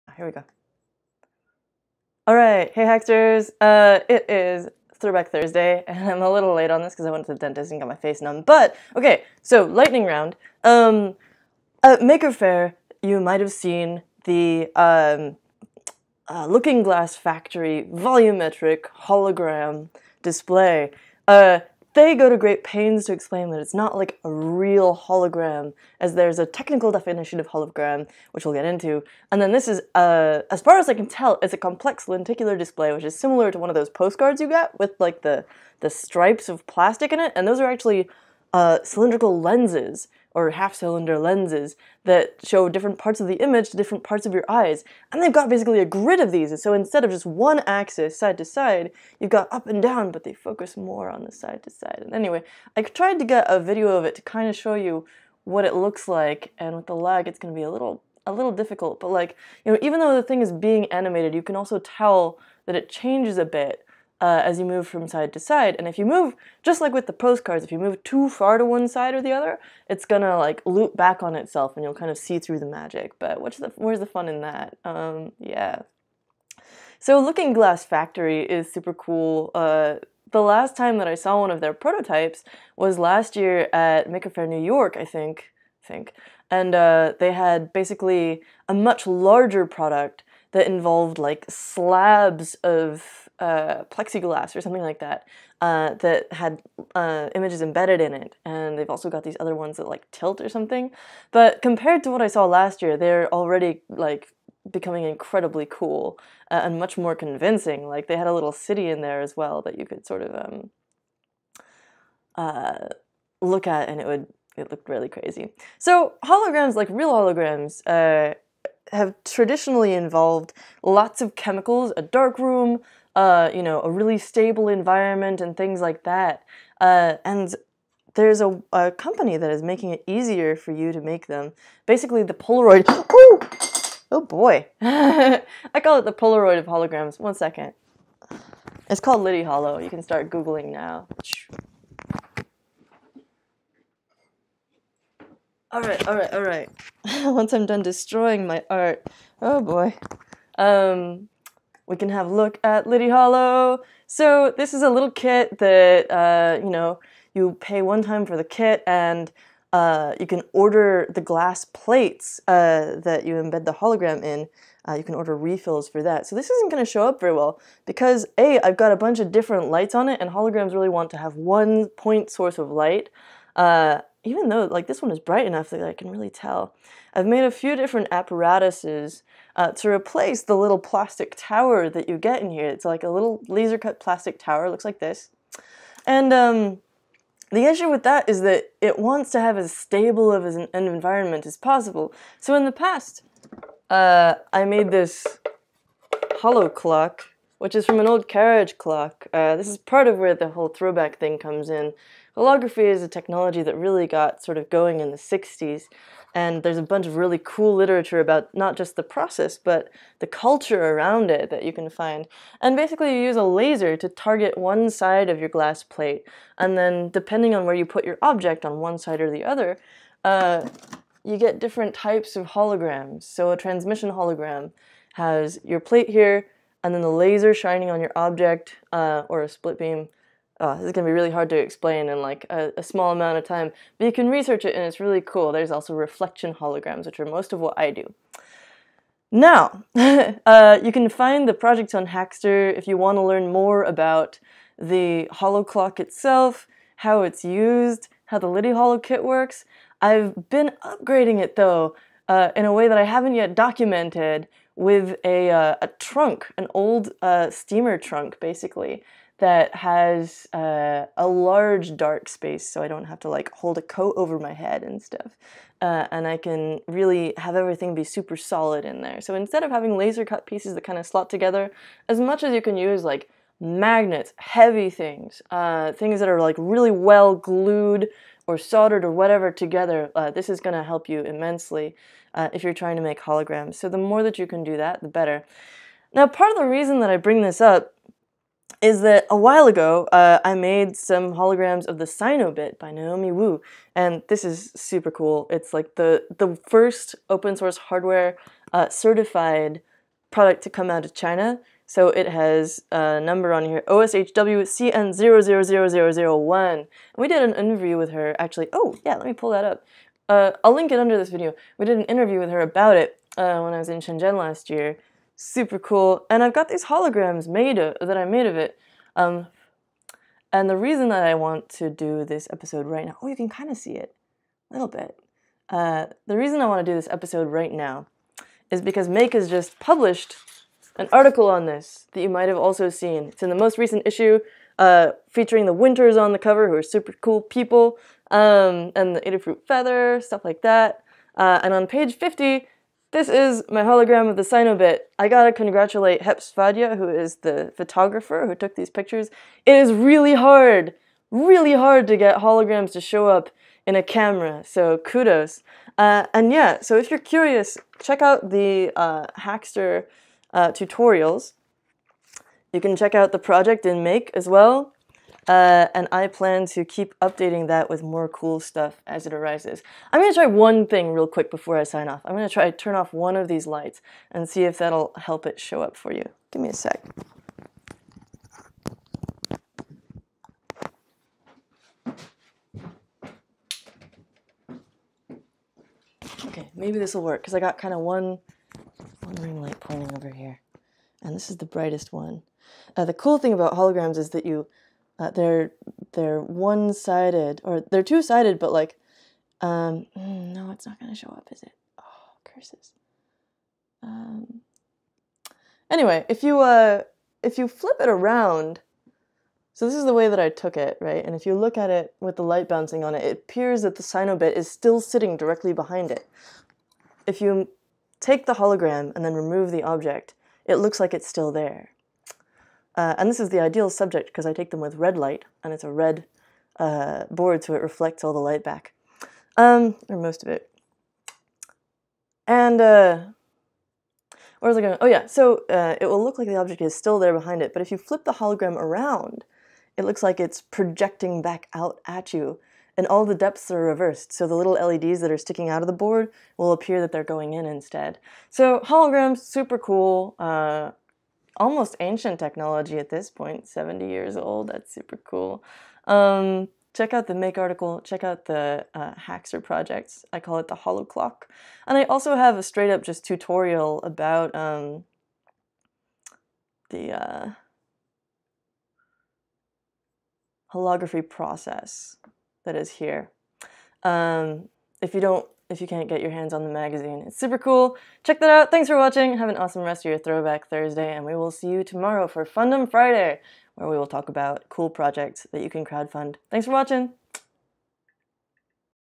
TBT: DIY Holography!
TBT: Holograms, "ancient" and modern! Feat. Looking Glass Factory, Make: Magazine, and Litiholo, as well as the Sino:bit by Naomi Wu and Elecrow!